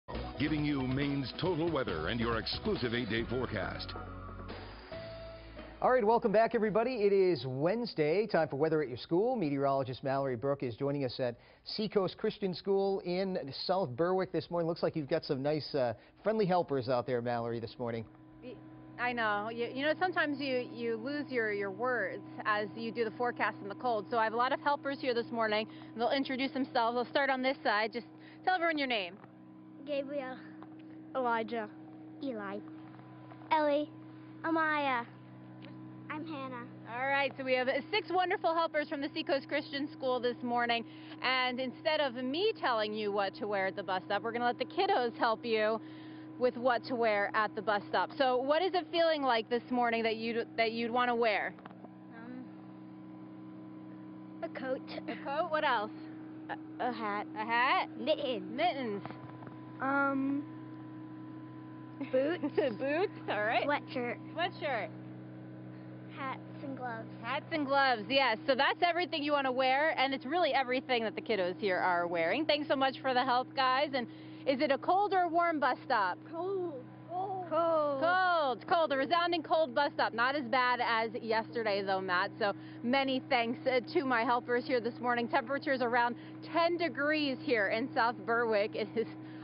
Seacoast Christian School Weather Report Feb 25, 2015 3rd Grade Students 1
WMTW Channel 8 came out to do the weather from Seacoast Christian